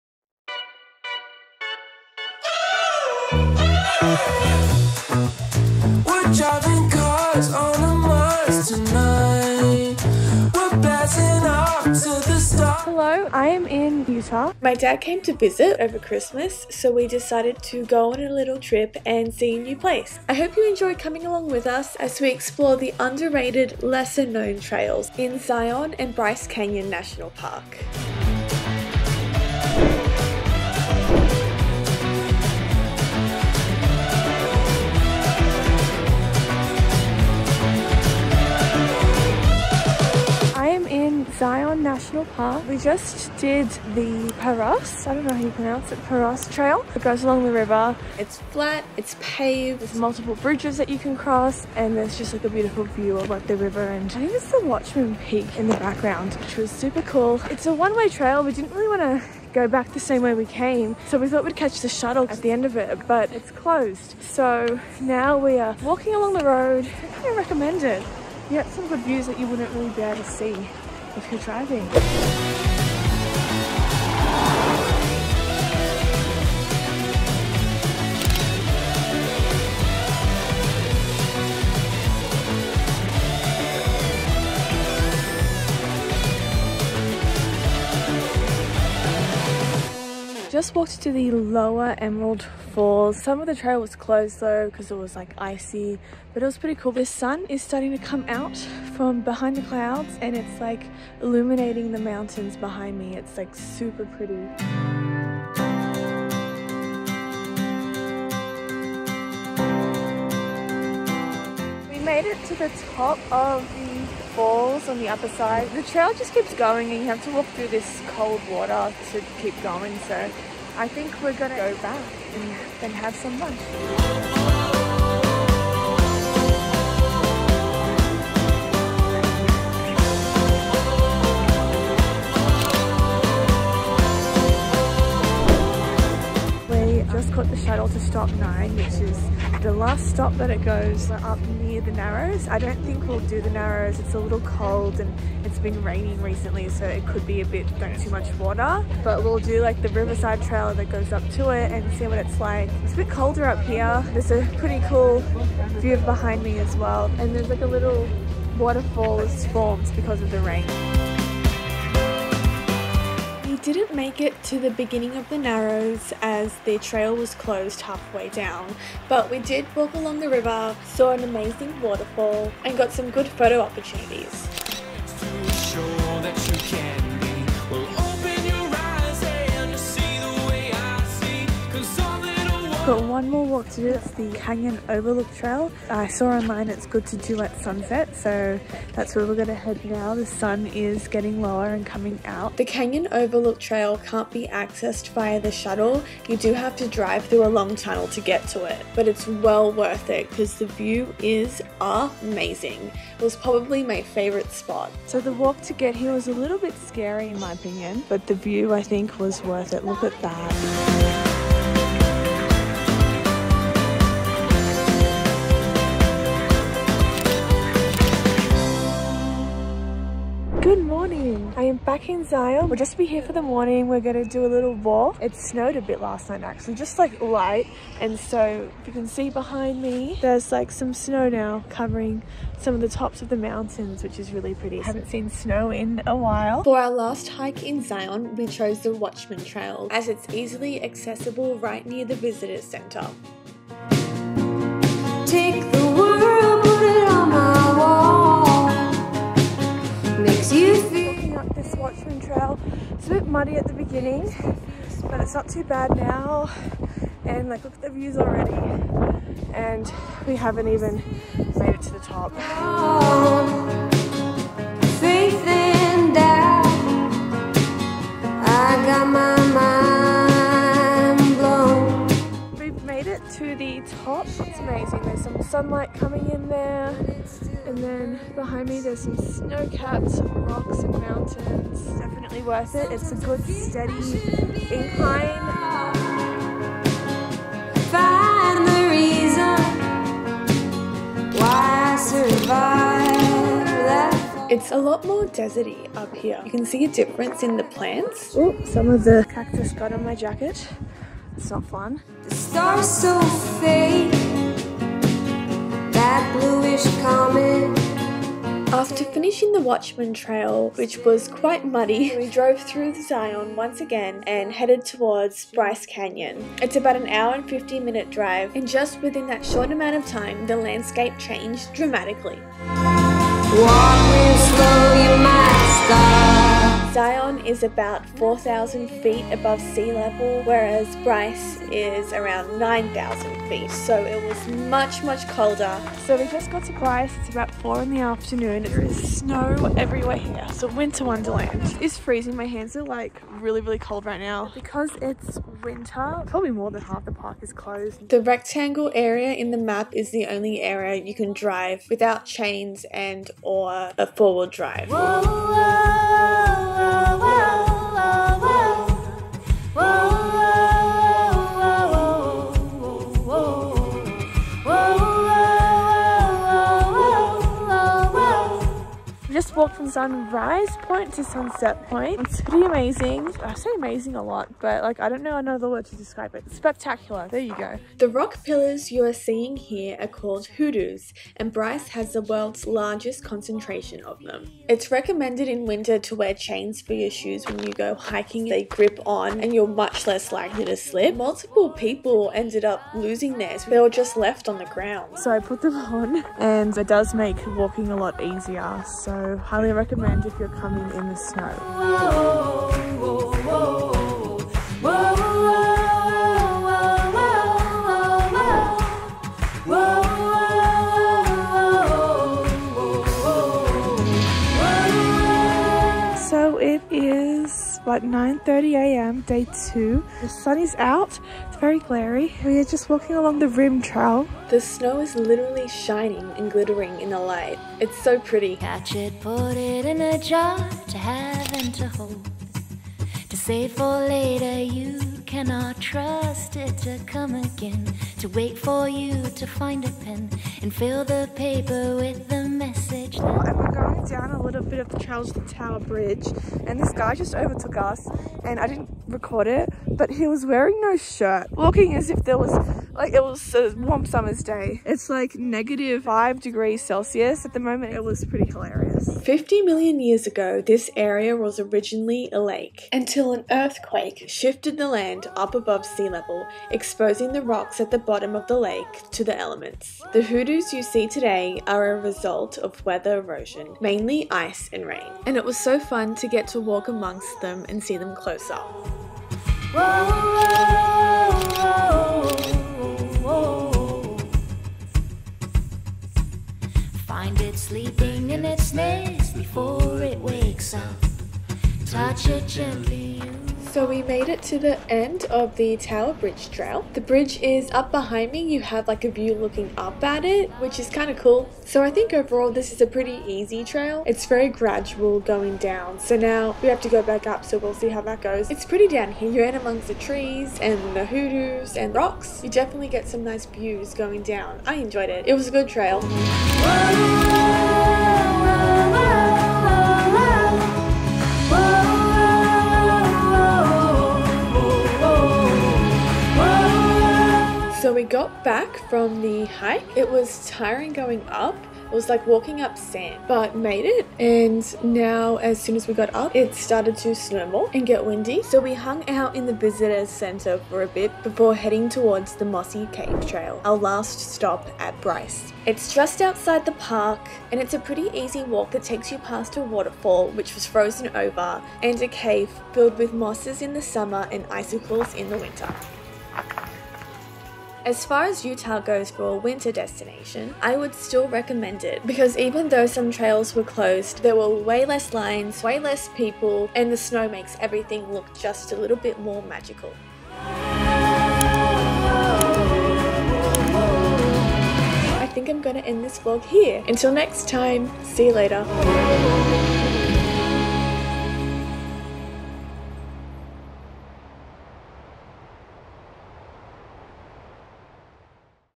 Zion and Bryce Canyon in Winter?
For a long time I had been wanting to visit the desert in Utah and in December when my Dad came to visit from Australia, I finally had the opportunity and we went on a father-daughter trip! Come with us as we explore some of the lesser known trails in Zion and Bryce Canyon national park in the off season! I definitely recommend coming here in the off season as lines for the shuttle and parking can be a nightmare in summer (or so I've heard!) What time of year is your favorite to travel?

Trails we did in Zion:
- Pa'rus trail (flat, easy, great views)
- Walking along the road..if it's not busy and a slow day at the park, could be worth it..but BE SAFE!!
- Lower Emerald Falls...there's 3 of them, lower, middle and upper. We didn't really noticed that much of a difference between the middle and the lower except for the angle of the waterfall..your pick!
- Riverside Walk
- Canyon Overlook Trail

Hikes to do on your next trip to Bryce Canyon:
- Rim trail
- Fairlyand Loop trail...but stopped at the tower bridge